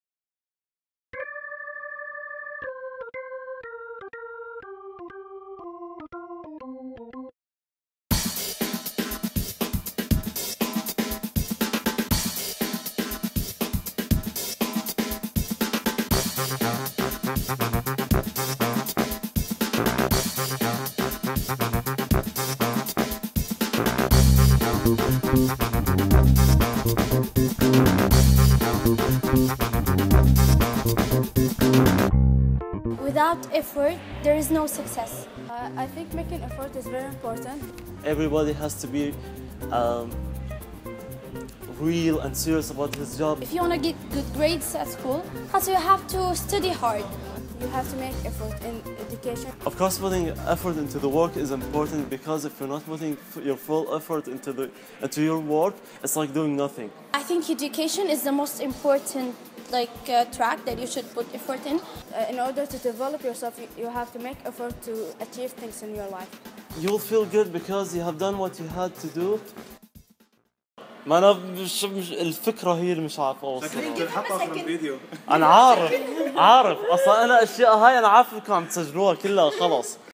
Day 4: Putting an Effort
Moza gave a class about History and Human rights followed by the auditions of Ta2heel's Got Talent!